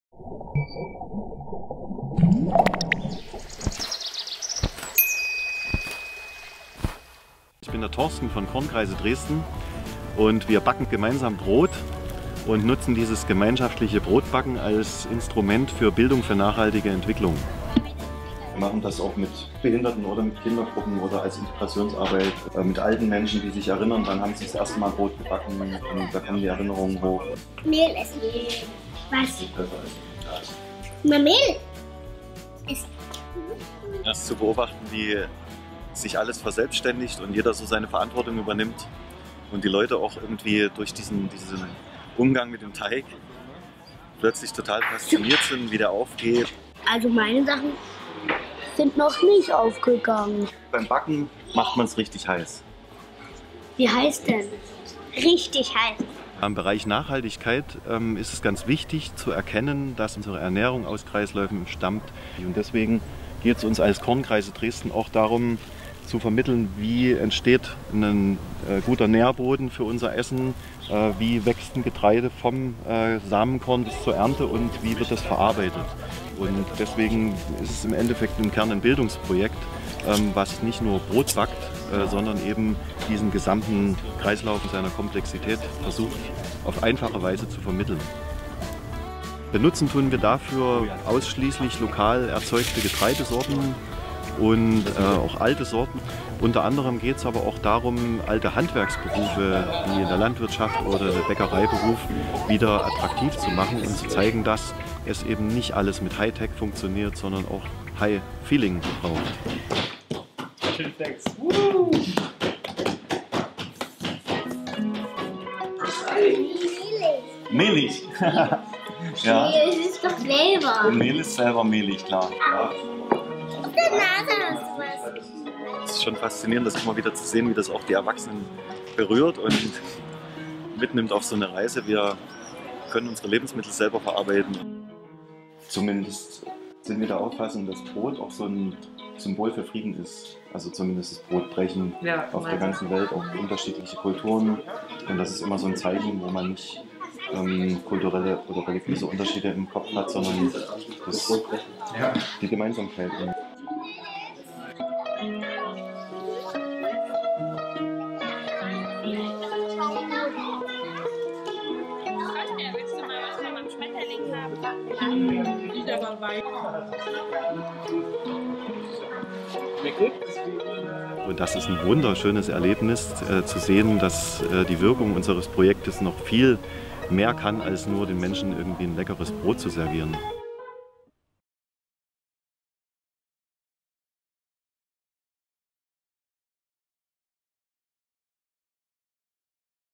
Kornkreise Dresden - Stories of Change
Die vielfältigen Kornkreise-Workshops verbinden die unterschiedlichsten Menschen im Sinne der Egalität miteinander und vermitteln ein breites Spektrum an Wissen für Jung und Alt. Daneben fördern sie die individuellen Gestaltungskompetenzen für eine nachhaltige Entwicklung des eigenen Lebens, der Gemeinschaft aller Menschen und der ganzen Welt, die uns umgibt.

Mehr als ein leckeres Brot.

So kann es vor allem ein erlebbares Beispiel für das Gelingen von Inklusion sein, denn was ist auf der Suche nach Gemeinsamkeiten in unterschiedlichen Kulturen schon vergleichbar mit Brot und mit dem Teilen dessen?